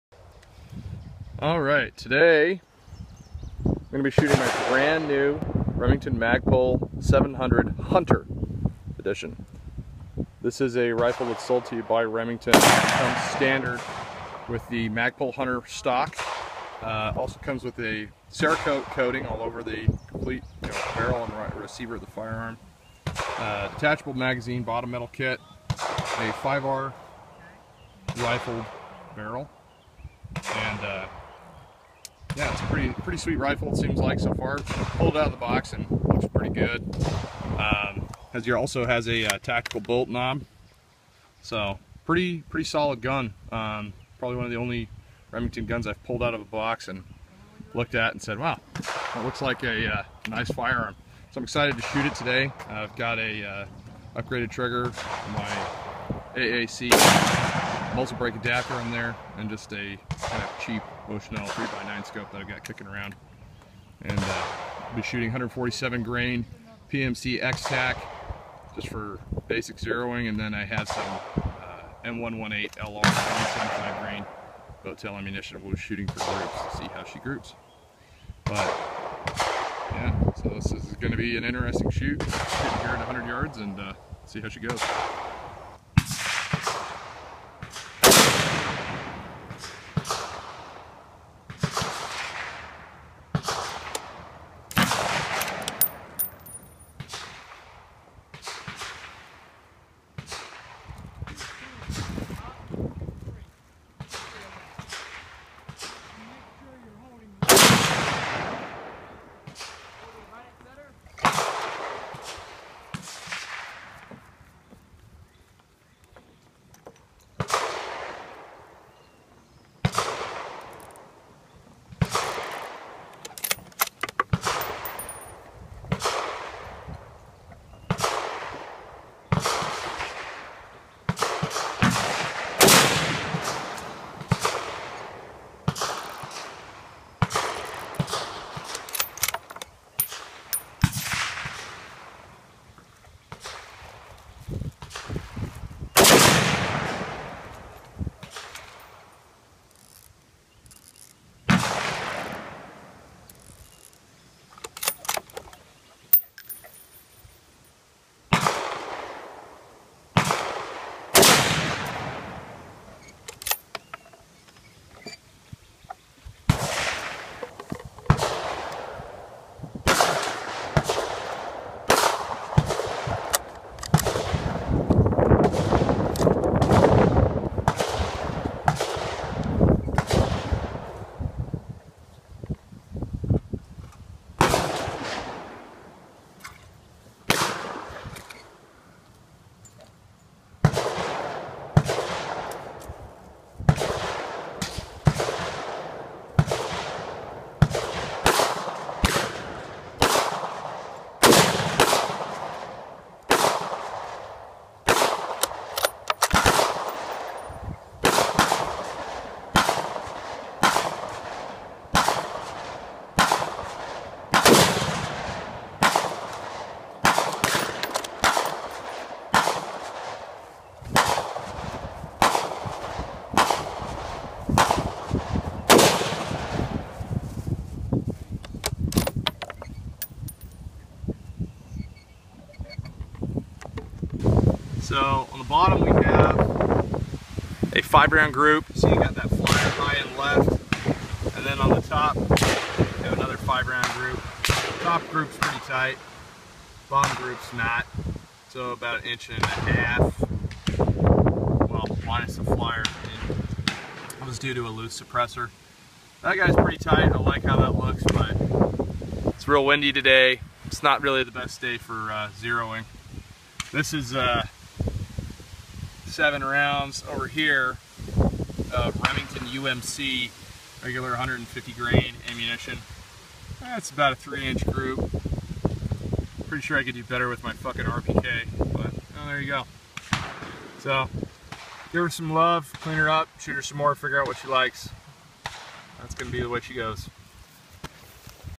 Remington 700 Magpul Hunter
Initial shooting of my new Remington Magpul edition 700. Initial accuracy was OK for a hot windy day and nothing special ball ammo.

Like the video? you might like a t-shirt